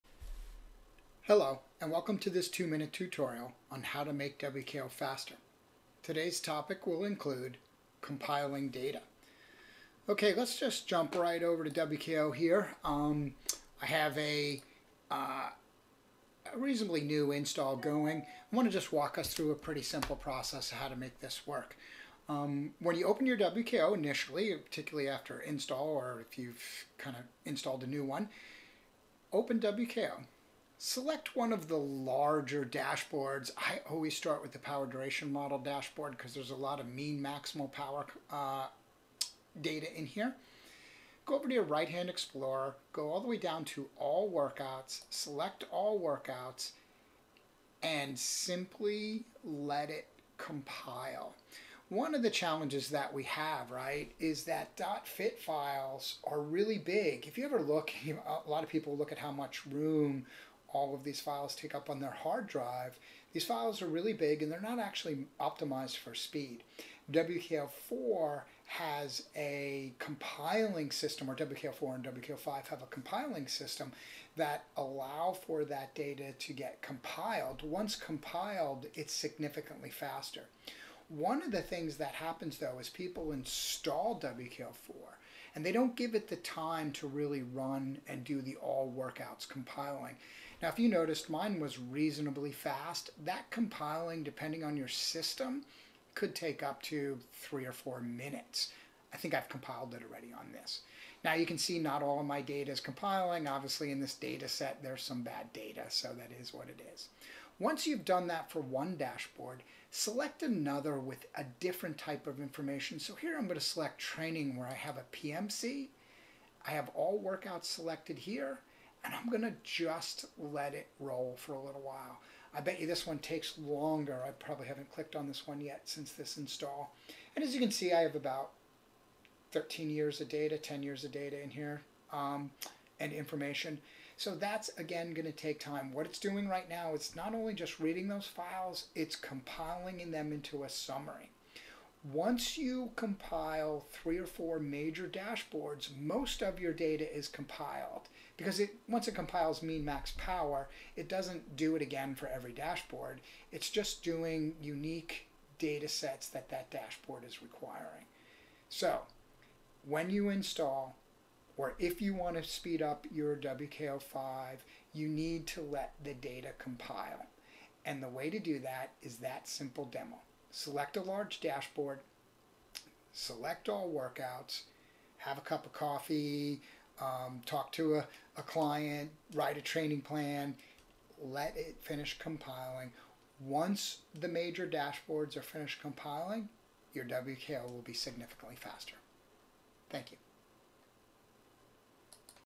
How to make WKO5 faster
This video demonstrates some ways to make WKO5 faster.

Not using WKO5?